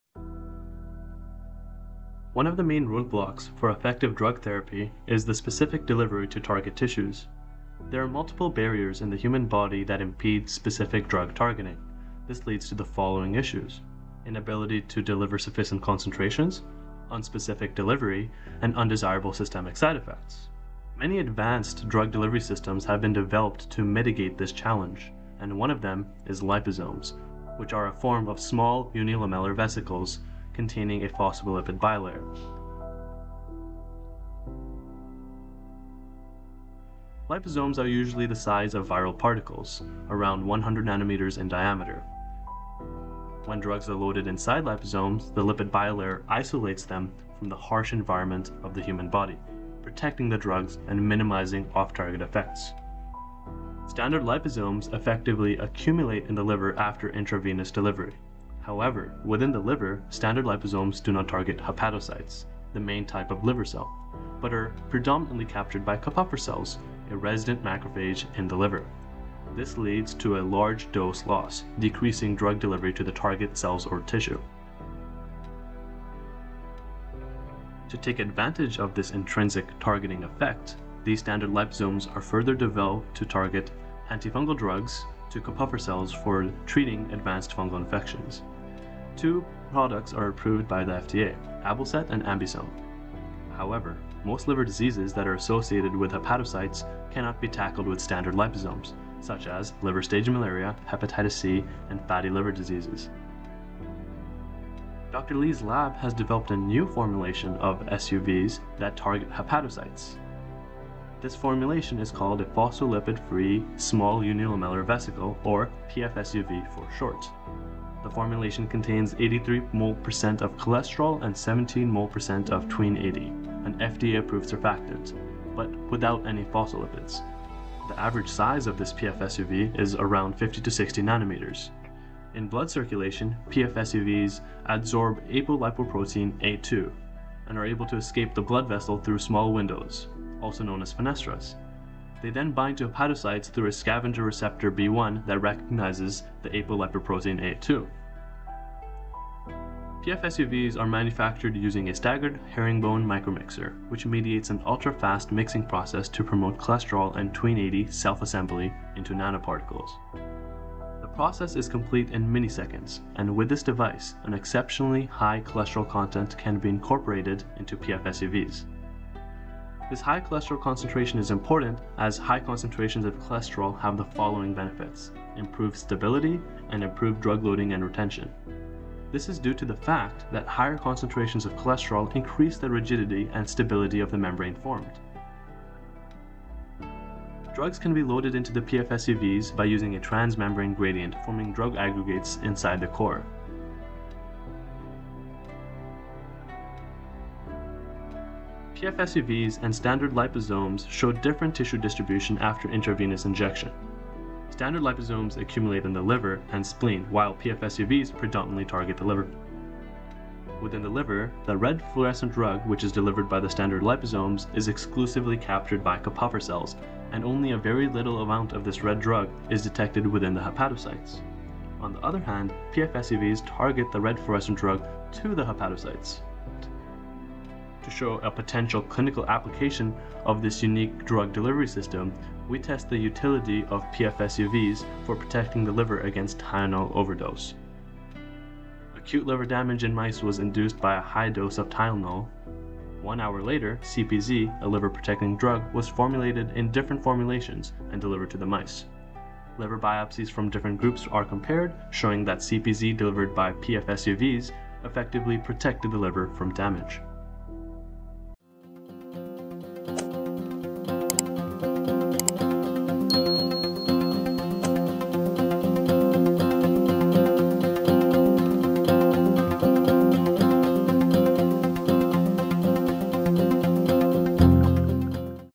Drug targeting to hepatocytes: pharmaceutical technology research and development - UBC Li Lab
Check out the Li Lab's recent research project: A highly innovative lipid nanoparticle formulation that predominantly targets the hepatocytes. Thanks to Rajeshwar Lally for creating this amazing animation to explain how this technology was developed, how it works for drug targeting, and its potential medical utilities. Must watch to the end!